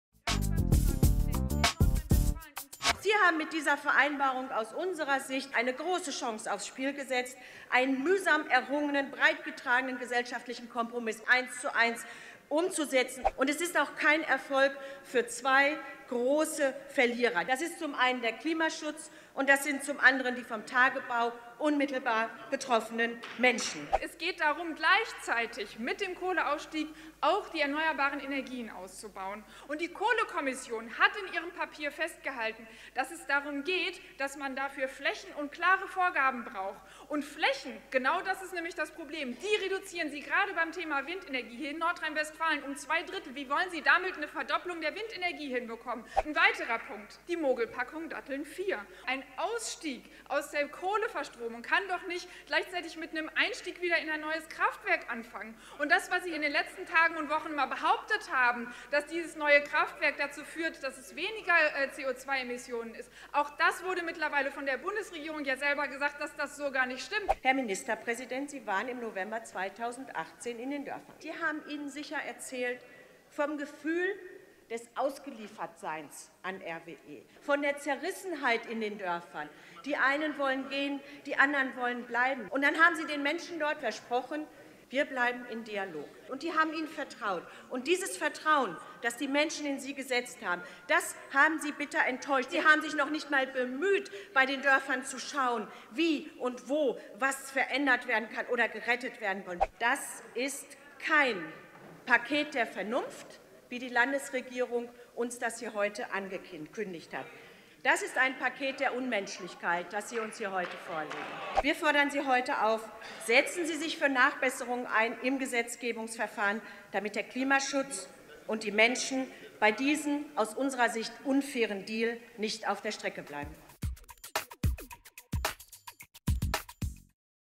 Monika Düker & Wibke Brems zur Debatte zum Kohleausstieg
Die Empfehlungen der Kohlekommission wurden nicht eins zu eins umgesetzt, die Klimaschutzziele in weitere Ferne gerückt & das Vertrauen der vom Tagebau unmittelbar betroffenen Menschen verspielt, resümieren unsere Fraktionsvorsitzende Monika Düker und Wibke Brems, unsere energiepolitische Sprecherin, heute in der Debatte zum Kohleausstieg im Landtag. Hier ihre Reden im Zusammenschnitt: